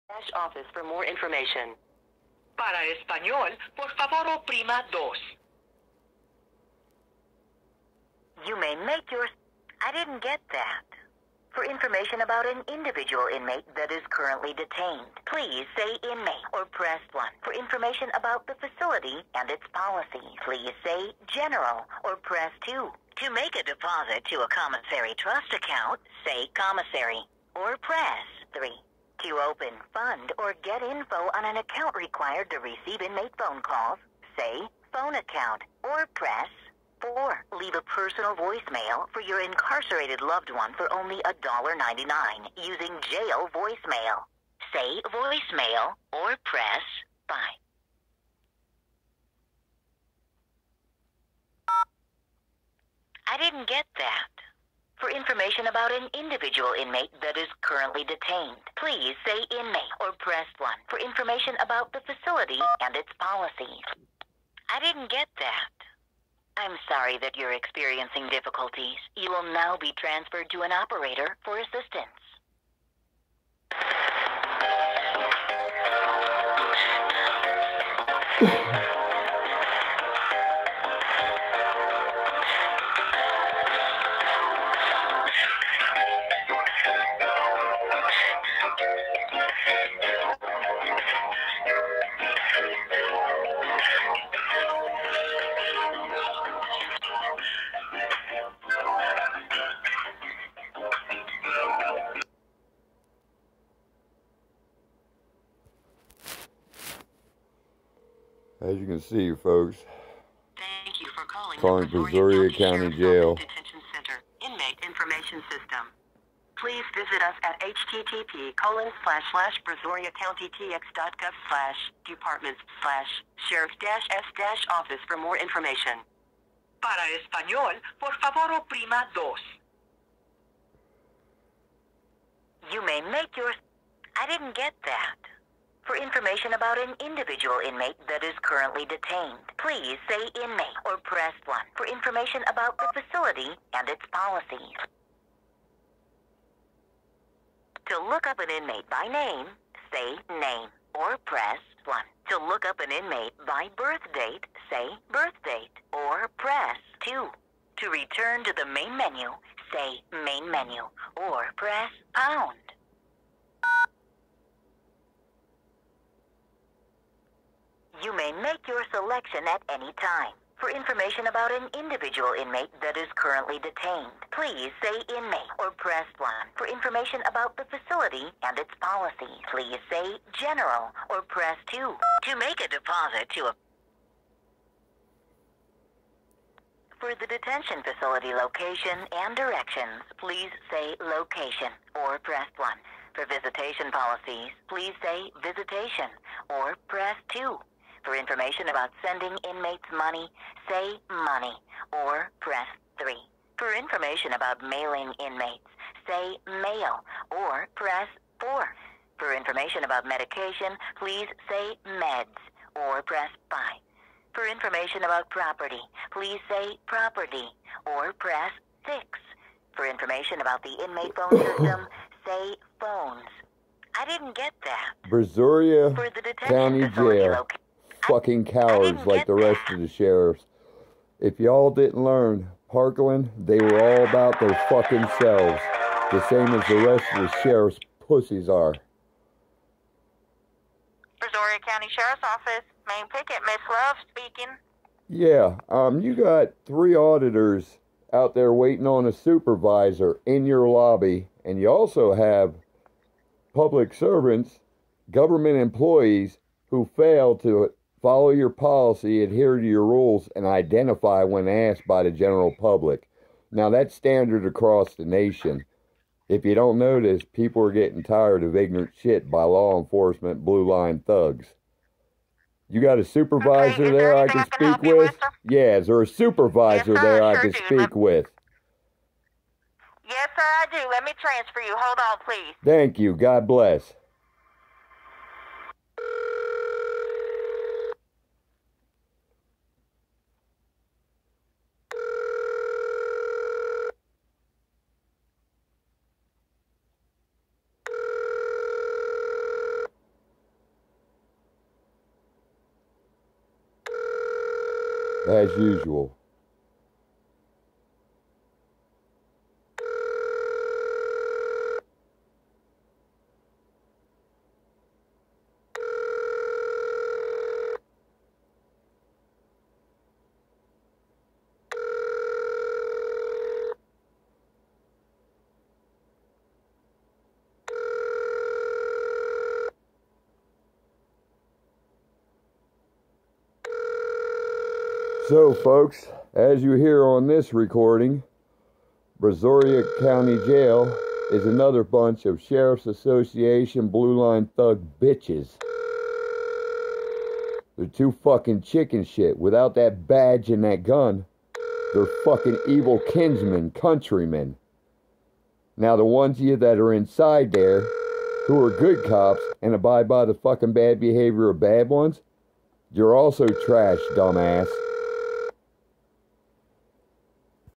1A auditor Jeff Grey arrested for 1st amdt protected activity. 9798642338 phones! 4 min. 20 sec. in
News Now Houston news now Patrick and Justin pull him trying to help Jeff Gray brazoria county jail refuses to give any information call their phones people light them up.!
start video around 4 minutes 20 seconds sorry I suck at editing.